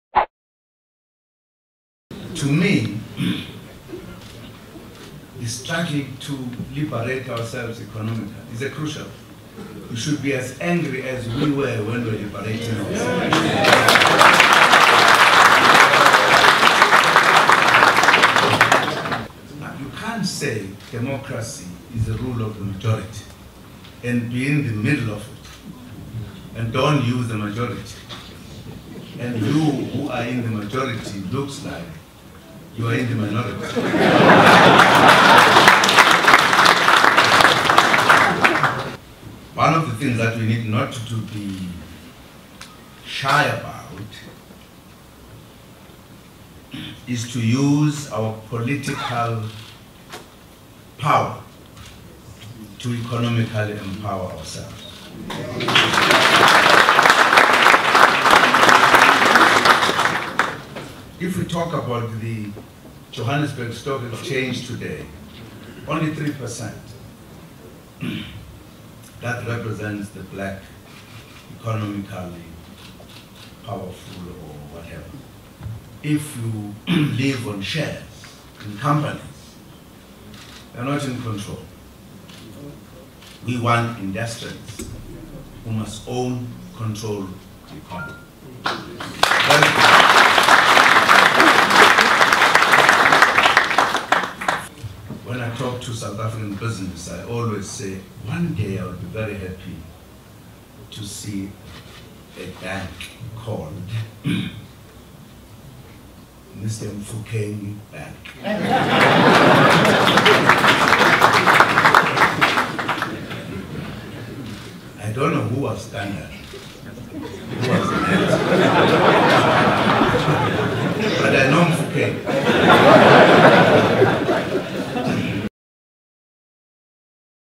Zimbabwe: Jacob Zuma in eie woorde
Die swart meerderheid moenie skaam wees om hul politieke mag vir ekonomiese gewin of bemagtiging te gebruik nie, het ’n strydlustige pres. Jacob Zuma Donderdag gesê.